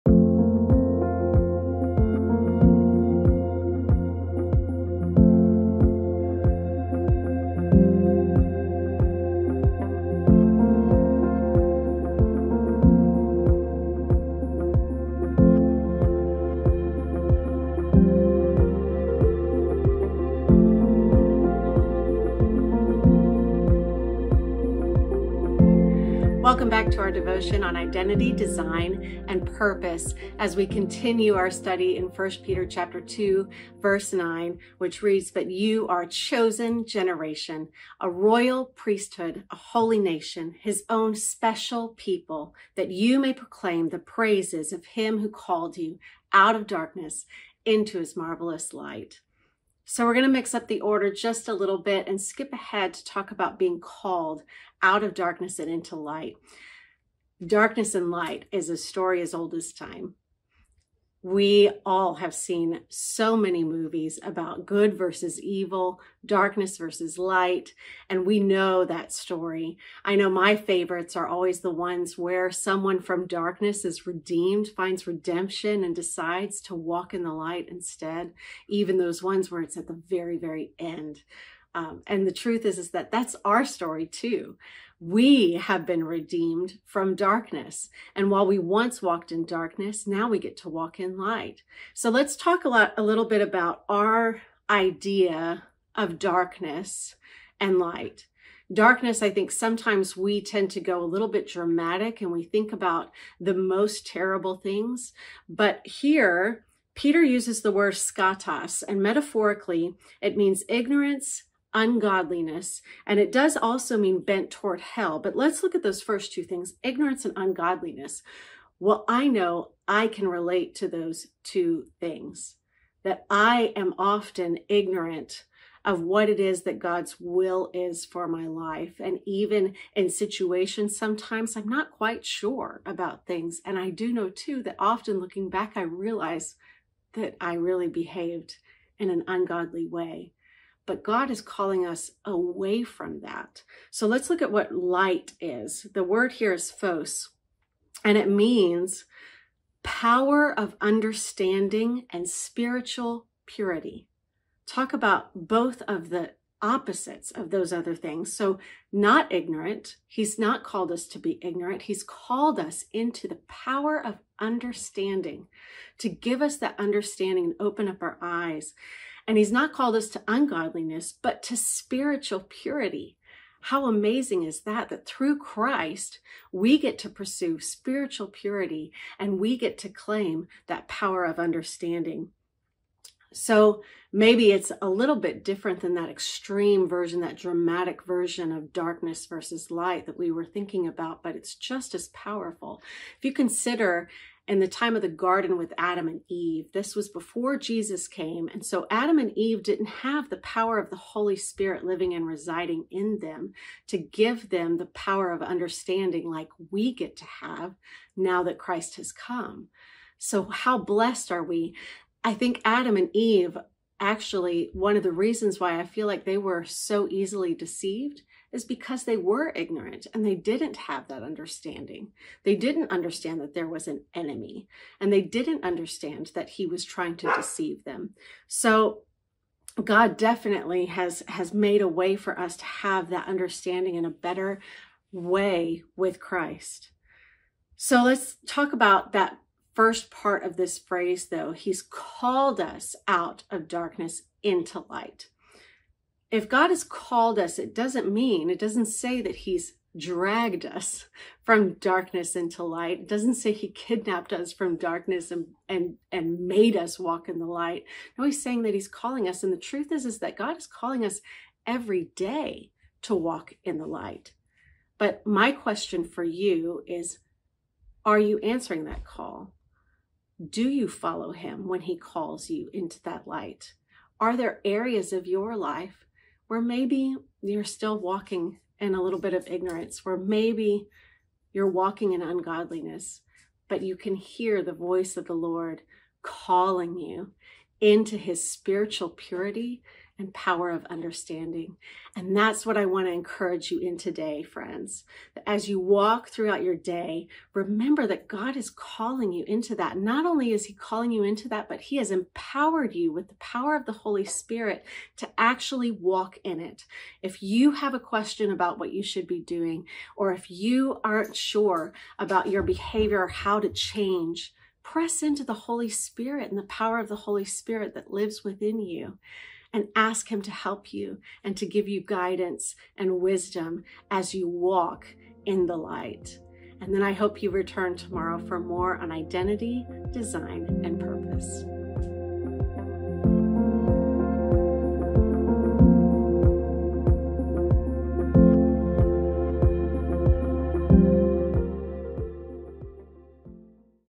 Identity, Design, Purpose - Daily Devotional / DAY 36
In today's devotion Worship Pastor Staci Pace continues sharing on I Peter 2:9.